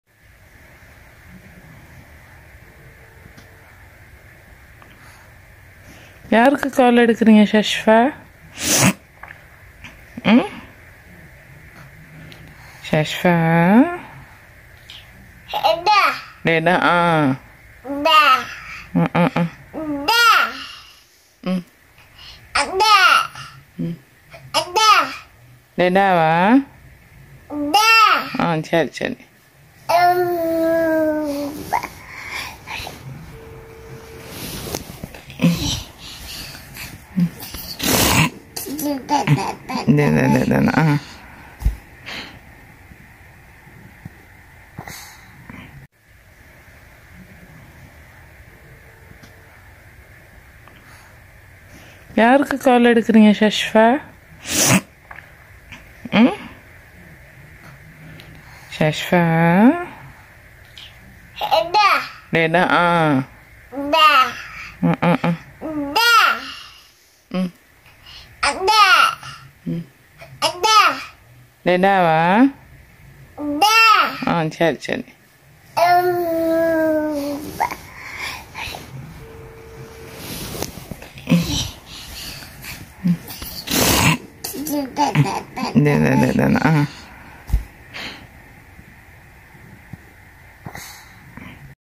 baby. After 1st birthday baby call to her father whenever ask mommy baby said dada#trending
please share if like